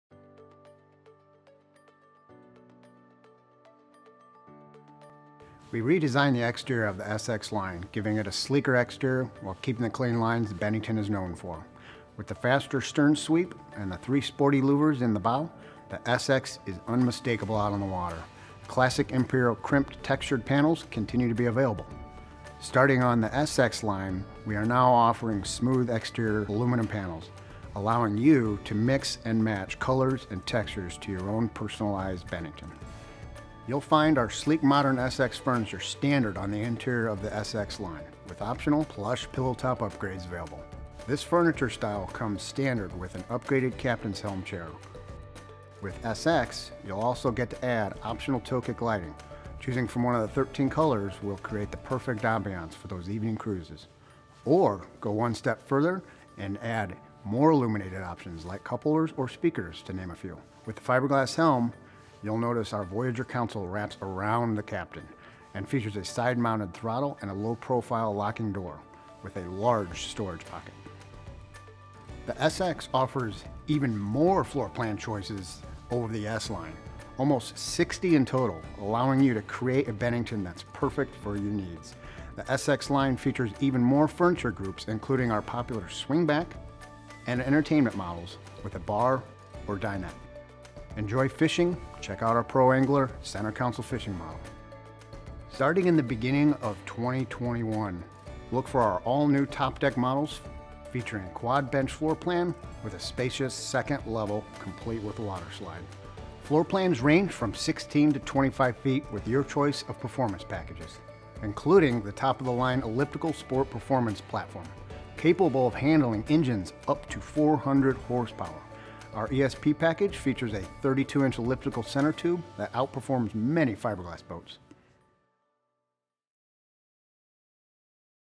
2021 Bennington SX Line of Pontoon Boats ☀️ What's New & Overview
The ever-popular SX gets a redesigned exterior for 2021 and still packs in many of our most popular standards, features, & upgrades.

We redesigned the exterior of the SX line giving it a sleeker exterior while keeping the clean lines Bennington is known for. With the faster stern sweep and 3 sporty louvers in the bow the SX is unmistakable out on the water. The classic imperial crimped textured panels continue to be available. Starting on the SX line we are now offering smooth exterior aluminum panels allowing you to mix and match colors and textures to own your ultimate personalized Bennington.

You'll find our sleek and modern SX furniture standard on the interior of our SX line with optional plush pillowtop upgrades available. This furniture style comes standard with an upgraded captain's helm chair. With SX, you'll also get to add optional toe kick lighting, choosing one of the 13 colors will create the perfect ambiance for those evening cruises, OR go one step further and add more illuminated options like cup holders or speakers, to name a few. With the fiberglass helm, you'll notice our Voyager console wraps around the captain and features a side-mounted throttle and a low-profile locking door with a large storage pocket.

The SX offers even more floorplan choices over the S line, almost 60 in total, allowing you to create a Bennington that's perfect for your needs. The SX line features even more furniture groups, including our popular swingback and entertainment models with a bar or dinette. Enjoy fishing, check out the pro angler center console fishing model.

Over the next few weeks look for our all-new top deck models featuring a quad bench floorplan with a spacious second level complete with a waterslide.

Floorplans range from 16 -25 feet with your choice of performance packages, including the top-of-the-line Elliptical Sport Performance platform capable of handling engines up to 400HP. Our ESP package features a 32" elliptical center tube that outperforms many fiberglass rivals.